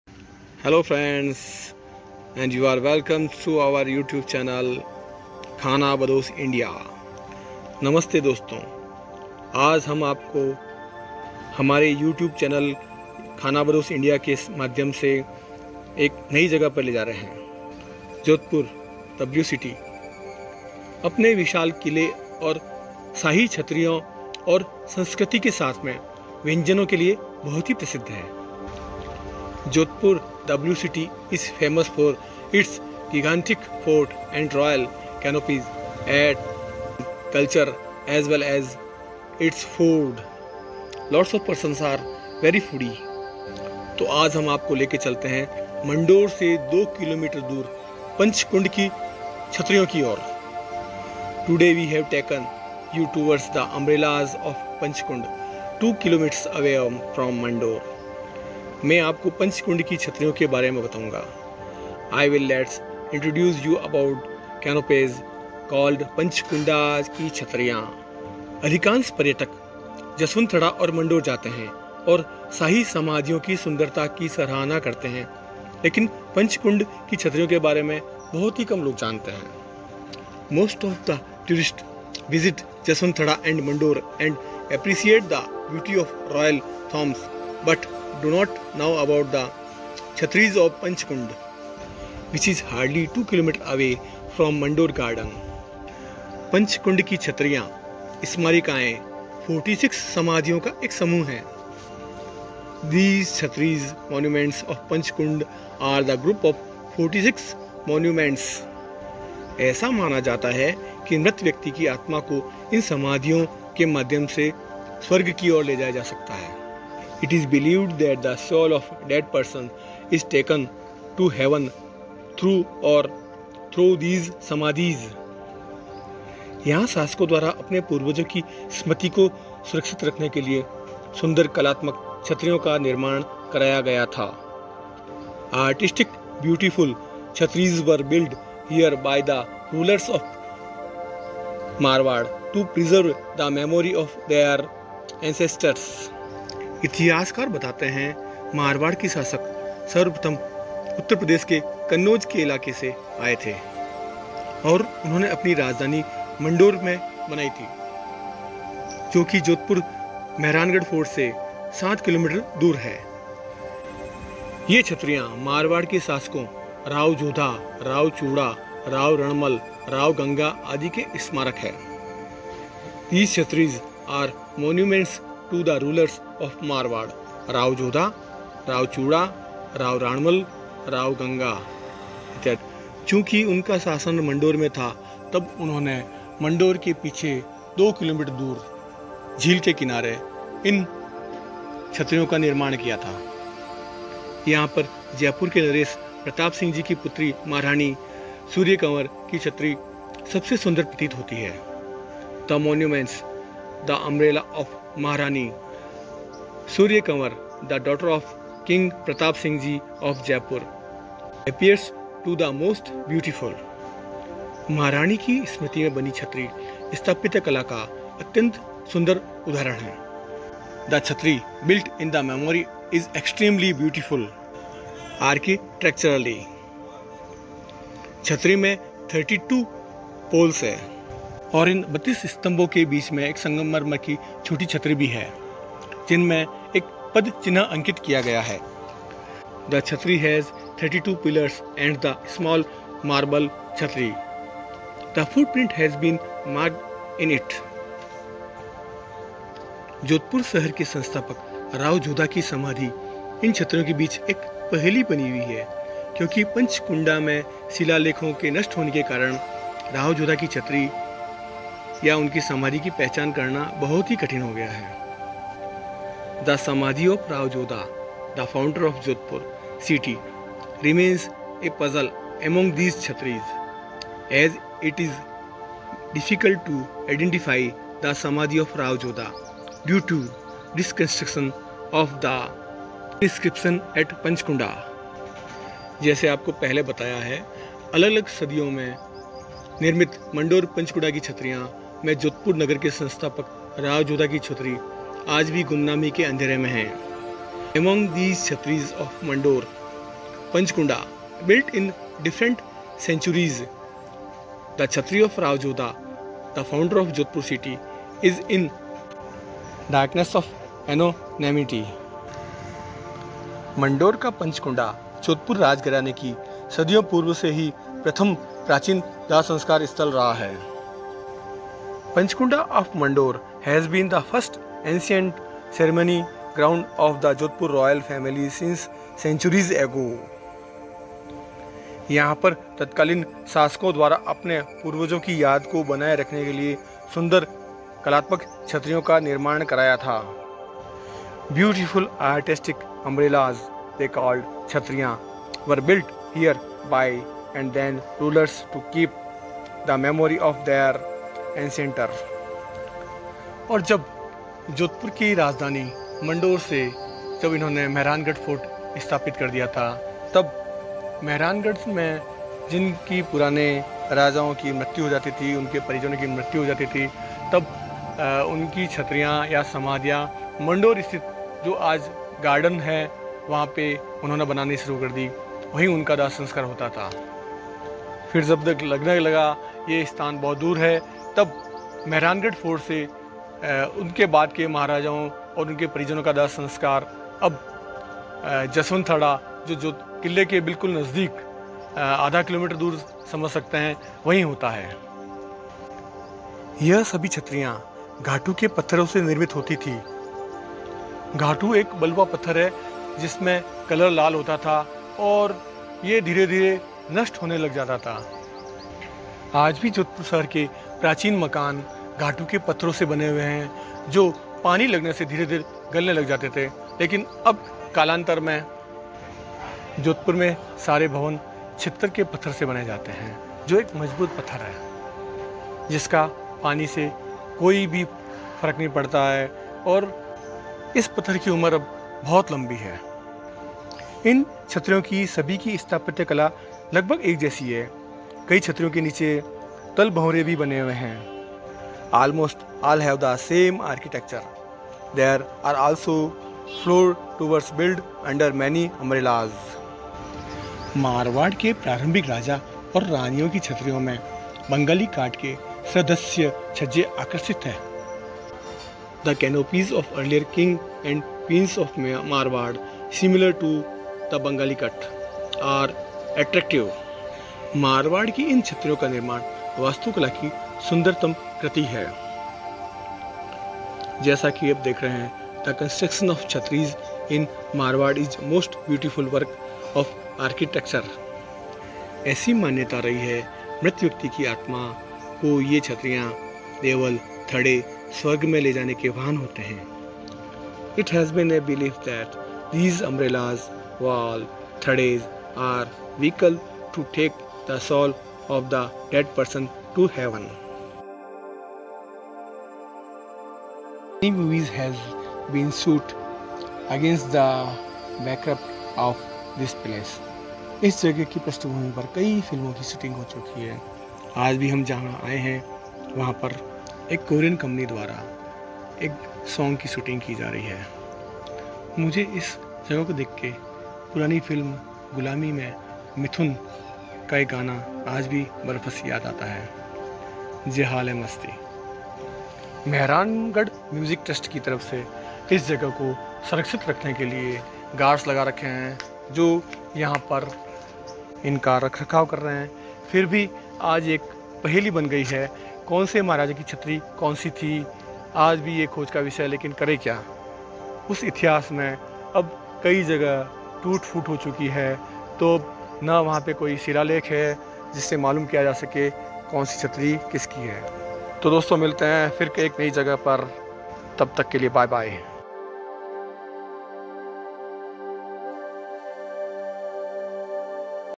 Panchkunda's Cenotaphs | पंचकुंड की छतरियां | Jodhpur | History | Monument | architecture
Panchkunda's Cenotaphs | पंचकुंड की छतरियां | Jodhpur | History |  Monument | architecture @blossomm_honey @JodhpurNewsexpress @YashHomeopathicCentreJodhpur @sterrino @devmusicjodhpuriya8127 @thehelpinghands3817 @SunCityMusicJodhpur @pawansafa7865 @RDCRajasthani @EnglishHeritage @indianheritager @TravelingDesi @mrfoodandtravel

ब्लू सिटी जोधपुर अपने विशाल किले और शाही छतरियों और संस्कृति के साथ-साथ व्यंजनों के लिए प्रसिद्ध है। आज हम आपको लेके चले हैं मदोर से 2 किलोमीटर दूर पंचकुंड की छतरियों की ओर | मैं आपको पंचकुंड की छतरियों के बारे में बताऊंगा |

Jodhpur the blue city is famous for its gigantic fort and royal cenotaphs and culture as well as cuisines. Today we have taken you towards the umbrellas of Panchkund, 2 km from Madore. I will let introduce you about cenotaphs called Panchkunda's cenotaphs ki chatris

अधिकांश पर्यटक जसवंथड़ा और मंडोर जाते हैं और शाही कब्रों की सुंदरता की सराहना करते हैं, लेकिन पंचकुंड की छतरियों के बारे में नहीं जानते हैं, जो मंडोर उद्यान से मुश्किल से 2 किमी दूर हैं।
Most of the tourists visit Jaswanthada and Mandore and appreciate the beauty of the royal tombs but do not know about the Chhatris of Panchkund. Which is hardly 2 km away from Mandore Garden

पंचकुंड की छतरियां (स्मारिकाएं) 46 समाधियों का एक समूह है। ऐसा माना जाता है कि मृत व्यक्ति की आत्मा को इन समाधियों या इसके माध्यम से स्वर्ग ले जाया जाता है।

The chhatris (monuments) of Panchkund are a group of 46 mausoleums. It is believed that the soul of a deceased person is taken to heaven through or through these samadhis.

यहां शासकों द्वारा अपने पूर्वजों की स्मृति को सुरक्षित रखने के लिए सुंदर कलात्मक छतरियों का निर्माण कराया गया था। छत्रियों के पास मारवाड़ के शासकों rao jodha  राव चूणादा, राव रणमल और राव गंगा आदि के स्मारक हैं।

Beautiful artistic chhatris were built here by the rulers to preserve the memory of their ancestors. Near Chhatriyas there are monuments to the rulers of Marwar, Rao Jodha, Rao Chunada, Rao Ranmal and Rao Ganga etc.

जयपुर के नरेश प्रताप सिंह जी की पुत्री महारानी सूर्यकांगर जी की छतरी सबसे सुंदर प्रतीत होती है।  उनकी स्मृति में बनी छतरी स्थापत्य कला से अत्यंत सुंदर है। छतरी में 32 स्तंभ हैं और संगमरमर की छोटी छतरी है। इसमें एक पदचिह्न अंकित किया गया है।

The umbrella of Maharani Suryakangar ji, daughter of King Pratap Singh ji of Jaipur, appears to be the most beautiful. The Chhatri built in his memory is extremely beautiful architecturally. The chhatri has 32 pillars and a small marble chhatri. A footprint has been marked in it.

जोधपुर शहर के संस्थापक राव जोधा की समाधि इन छतरियों के बीच पहेली बनी हुई है क्योंकि पचकुंडा में शिलालेखों के नष्ट होने के कारण राव जोधा की समाधि की पहचान करना कठिन है।

The Samadhi of Rao Jodha, the founder of Jodhpur city, remains a puzzle among these chhatris as it is difficult to identify the Samadhi of Rao Jodha due to the destruction of the inscriptions at Pachkunda.

अलग-अलग सदियों में निर्मित मंडोर पंचकुंडा की छतरियों में जोधपुर नगर के संस्थापक राव जोधा की छतरी गुमनामी के अंधेरे में है। मंडोर का पंचकुंडा जोधपुर राजघराने का सदियों पूर्व से ही प्रथम प्राचीन दाह-संस्कार स्थल रहा है। यहां पर तत्कालीन शासकों द्वारा अपने पूर्वजों की याद को बनाए रखने के लिए सुंदर कलात्मक छतरियों का निर्माण करवाया गया।

Among the Chhatris of Mandore Panchkunda built in different centuries, the Chhatri of Rao Jodha, the founder of Jodhpur city, is in the darkness of anonymity., Panchkunda of Mandore has been the first ancient cremation ground of the Jodhpur royal family since centuries ago. Beautiful artistic umbrellas were built here by the then rulers to keep the memory of their ancestors.

सभी छतरियां घाटू के पत्थरों से निर्मित हैं। लगभग सभी की स्थापत्य कला एक सी है। कई छतरियों के नीचे तल-भवंरे भी बने हुए हैं। मारवाड़ के प्रारम्भिक राजा व रानियों कीछतरियों में बंगाली काट के सदृश्य छज्जे आकर्षक हैं। मारवाड़ में छतरियों का निर्माण वास्तुकला की सुन्दरतम कृति हैं। ऐसी मान्यता रही है कि मृत व्यक्ति की आत्मा को ये छतरियां, देवल, थड़े स्वर्ग में ले जाने के वाहन होते हैं।
इस जगह की पृष्ठभूमि पर कई फिल्में शूट की गई हैं।

All the umbrellas are made of Ghatu stones.  Almost all have the same architecture. There are also floor-towers built under many umbrellas.

Many movies have been shot against the backdrop of this place.